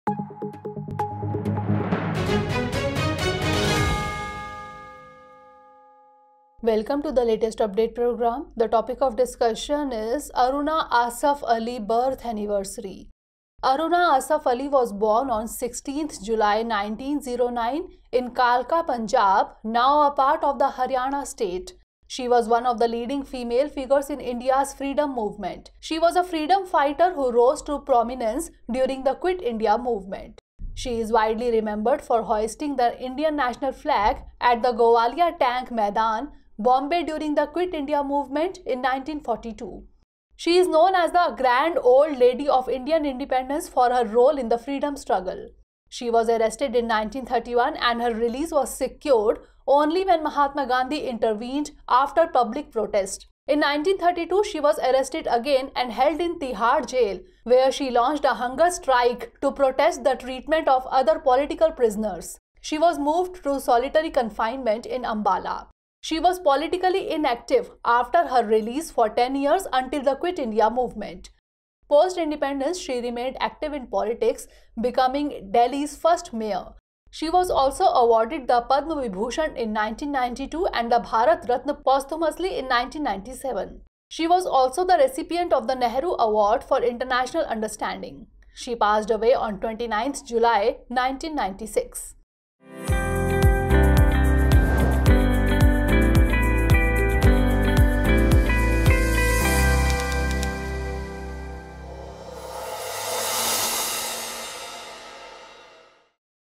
Aruna Asaf Ali Birth Anniversary: Latest update | Drishti IAS English
Aruna Asaf Ali was born on 16 July 1909, in Kalka Punjab (now a part of the Haryana state). She was one of the leading female figures in India’s freedom movement. She was a freedom fighter who rose to prominence during the Quit India Movement..